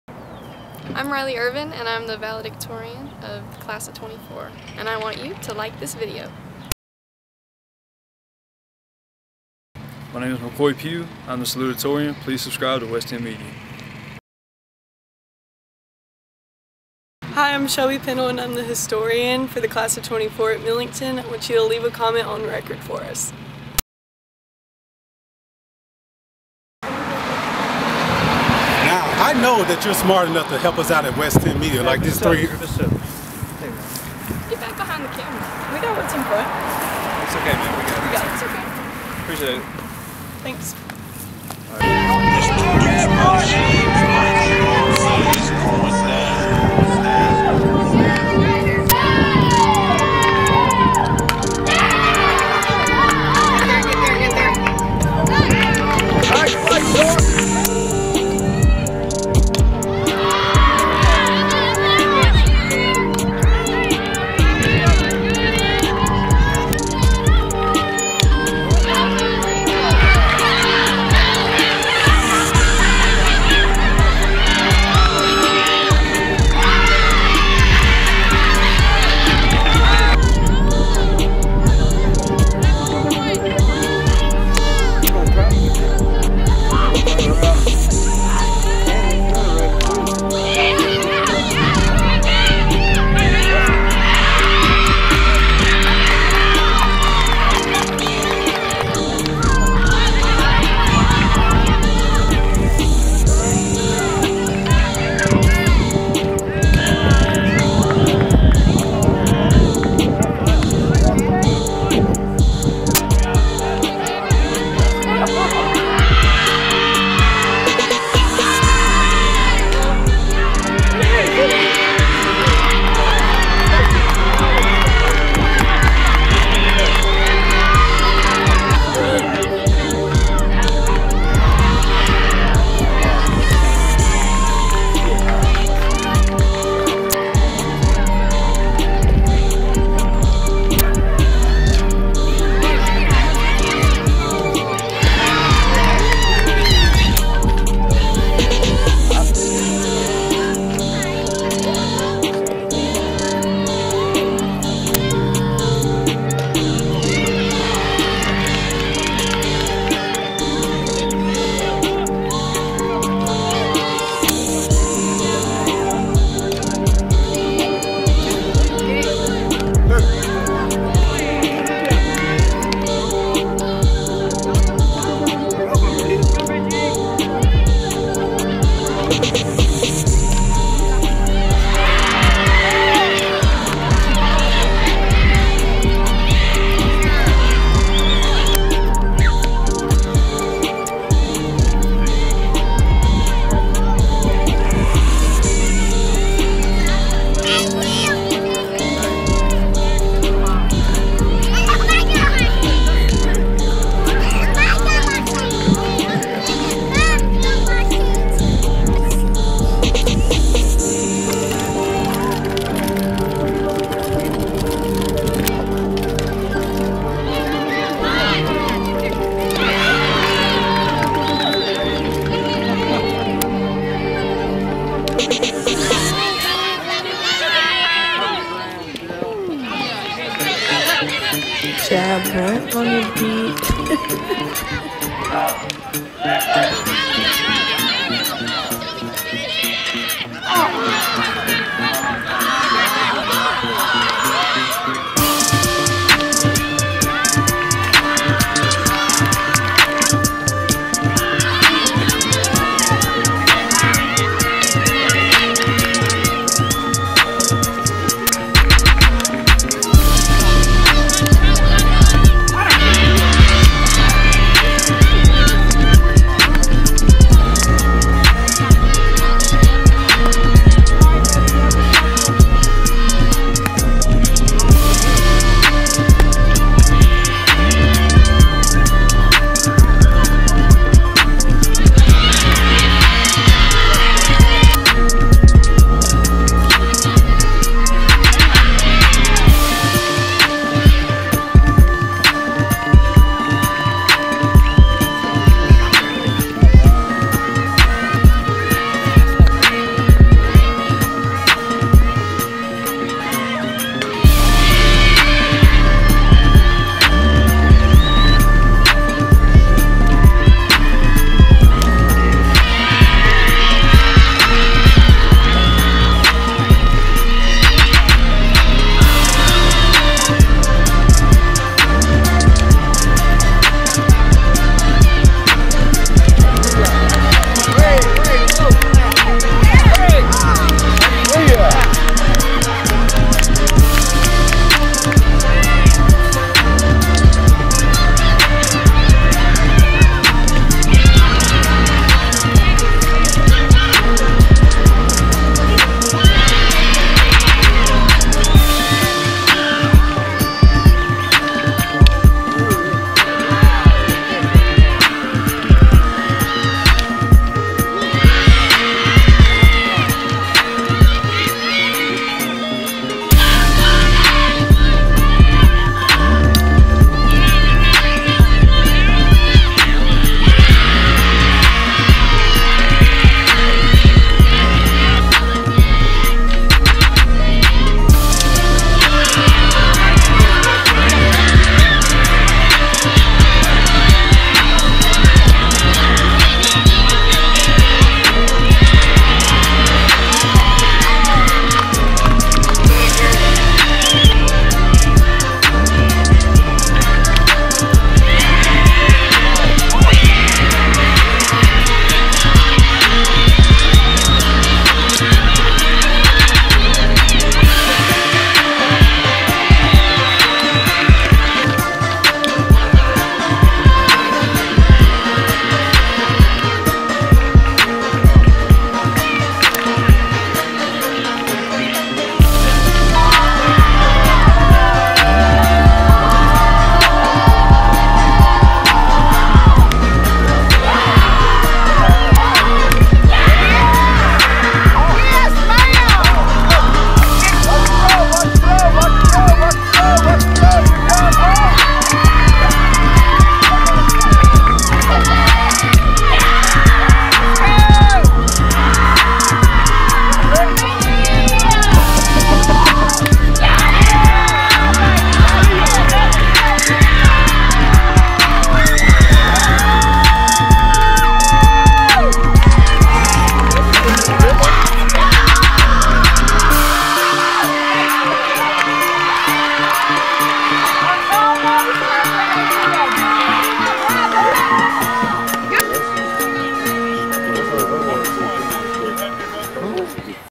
2024 TSSAA Softball Munford @ Millington
SOFTBALL: Munford 22, Millington 15
Normally when a team scores 15 runs and holds a 12-3 advantage at one point of a softball game, that head coach leaves the field satisfied and victorious.
Millington Lady Trojan Skipper Kasey Craddock exited her home field Thursday night puzzled because rival and now district foe the Munford Lady Cougars earned a 22-15 victory with 19-0 run at one stretch.
“They just picked up on our pitching, even when we changed it,” Craddock noted. “They scored a few and got momentum. We made some major errors and then they were hitting them where we couldn’t catch them with people on base.
“It was definitely the perfect storm,” she added. “We had pitch charts and all of that. They were just seeing it.”
Millington was seeing Munford pitching well in the early innings grabbing a 3-2 lead in the first inning. Lady Trojan Riley Washburn had a RBI double and Shelby Pindell singled in the go-ahead run.
The Lady Trojans surged ahead 5-2 in the second inning featuring a Kaleigh Wilston RBI base hit.
After the Lady Cougars stretched out a run in the top of the third, Millington put together another three-run frame. Breanna Hulsey drove in a pair of runners with a single.
Then the Lady Trojans seemed to put the game away in the fourth inning with four runs highlighted by a Grace Bell RBI triple off the wall. The Lady Cougars faced a 12-3 deficit heading into the fifth inning and a crossroad.